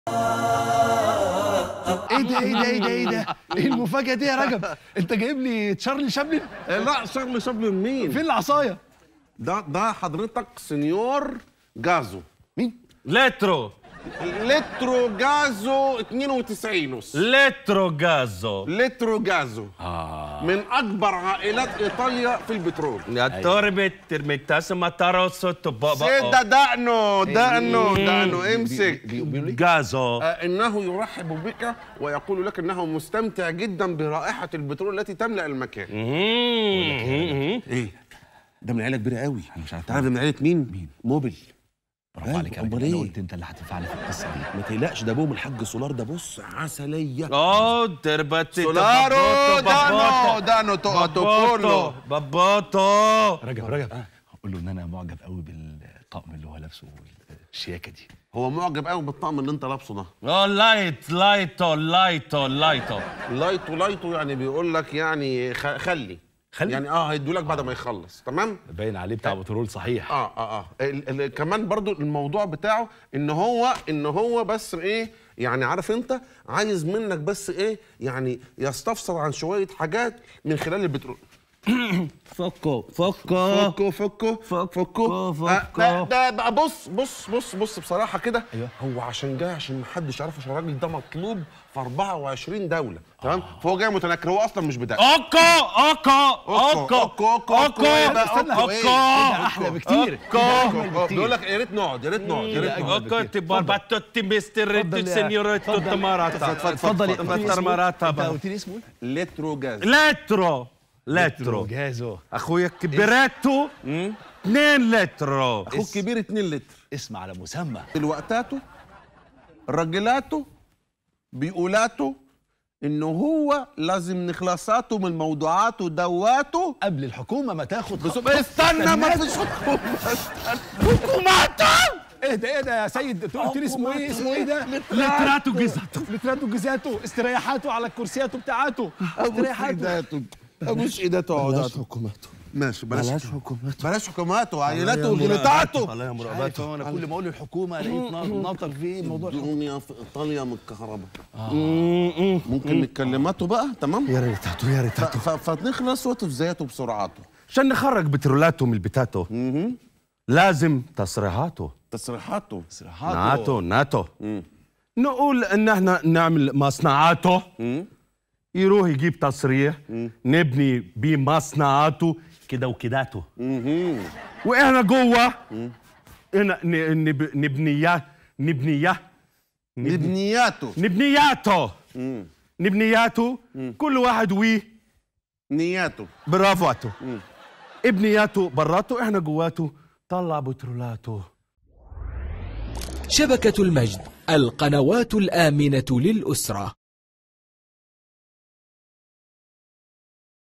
سوا نفسه خبير إيطالي وقاعد يجيب العيد في اللهجة والمصطلحات "بترولاتو" 😂🏃‍♂️
على خشبة المسرح
أدوار وقصص مختلفة

في برنامج